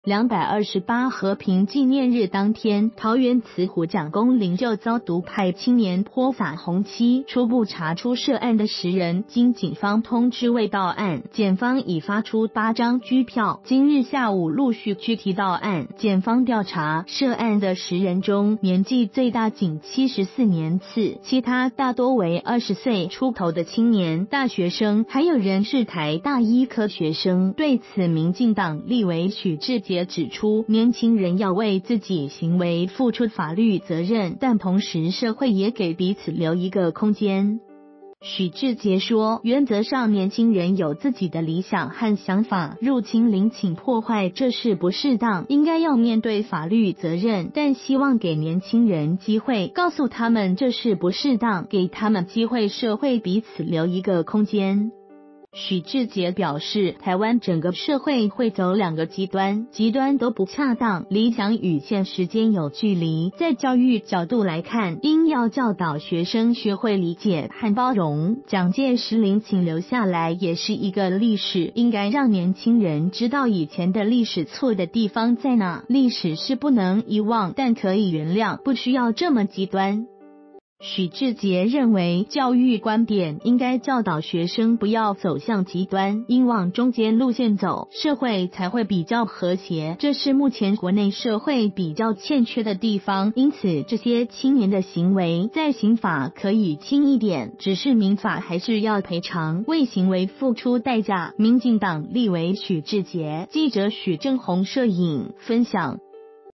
獨派青年蔣公陵寢潑漆遭逮 綠委：給年輕人機會與空間
228和平紀念日當天，桃園慈湖蔣公靈柩遭獨派青年潑灑紅漆，初步查出涉案的10人經警方通知未到案，檢方已發出8張拘票，今日下午陸續拘提到案。檢方調查，涉案的10人中，年紀最大僅74年次，其他大多為20歲出頭的青年、大學生，還有人是台大醫科學生。對此民進黨立委許智傑指出，年輕人要為自己行為付出法律責任，但同時社會也給彼此留一個空間。